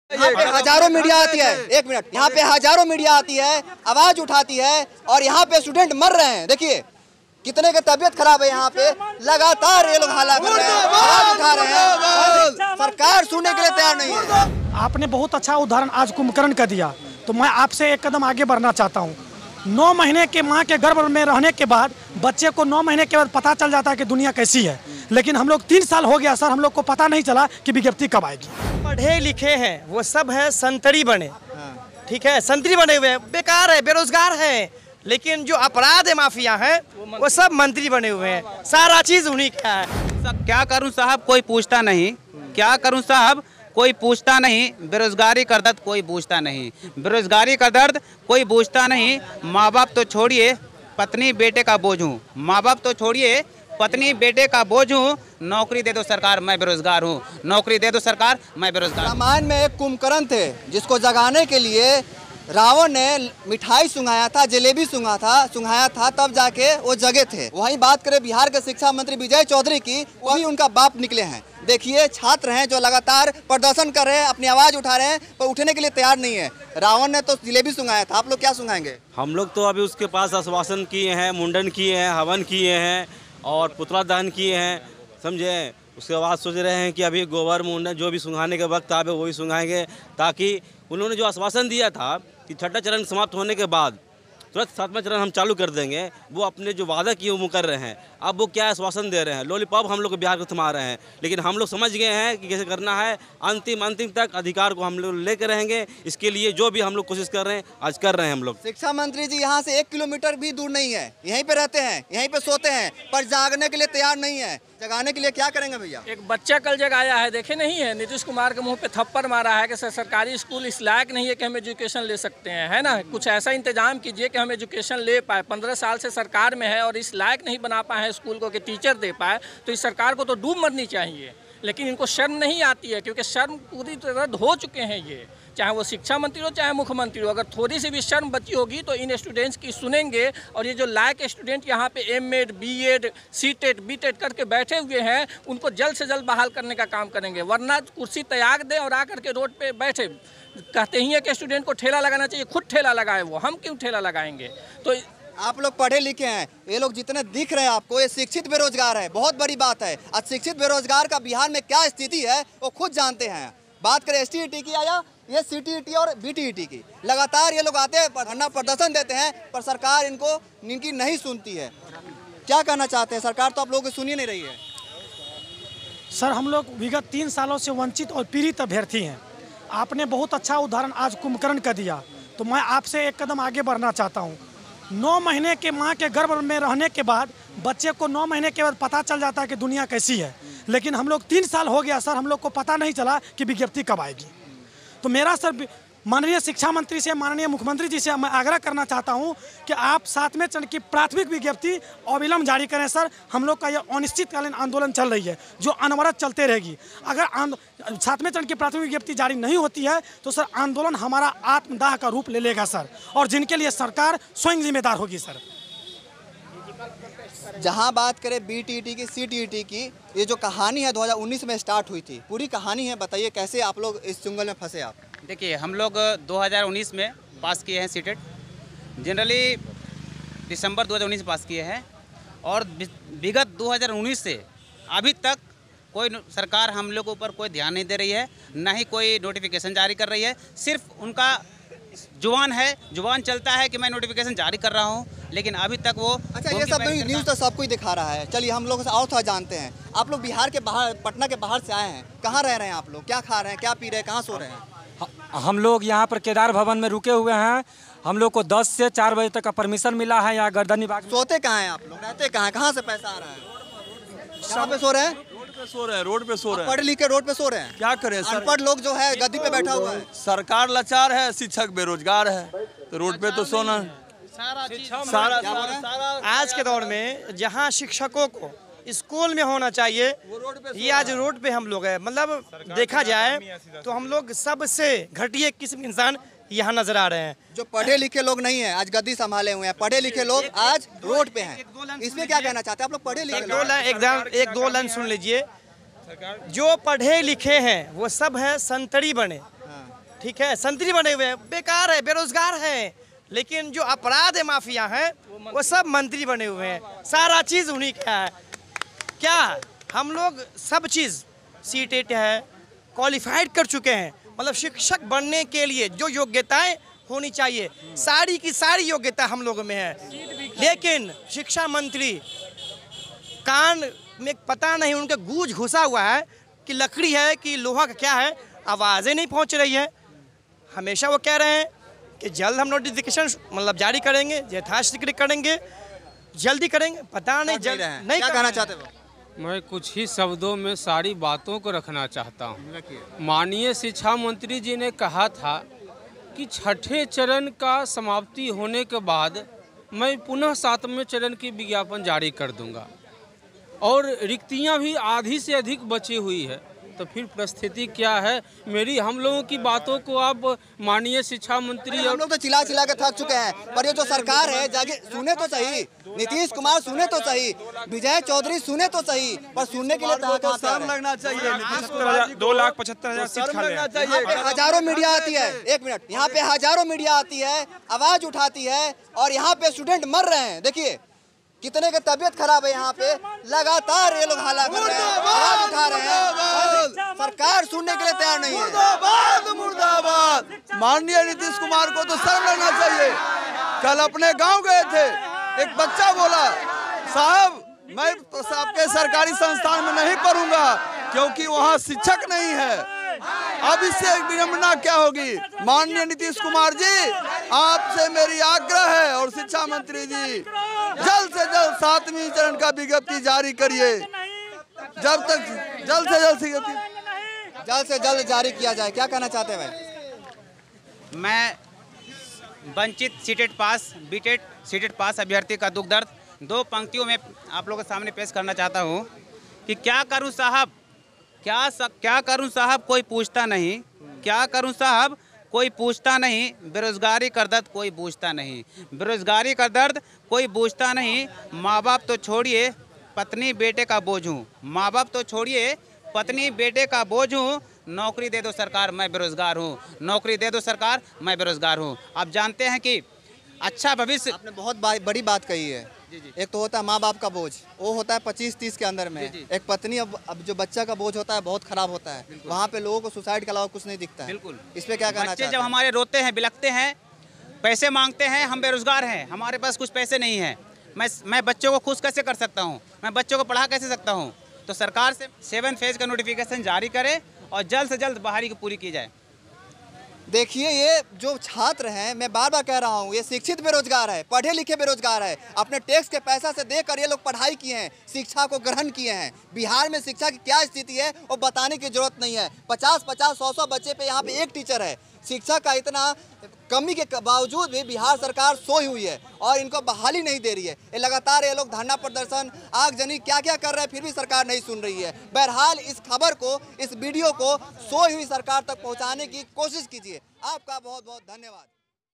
CTET/BTET आंदोलन में बोले छात्र-मर जायेंगे पर झुकेंगे नही
ctet andolan, ctet stet andolan,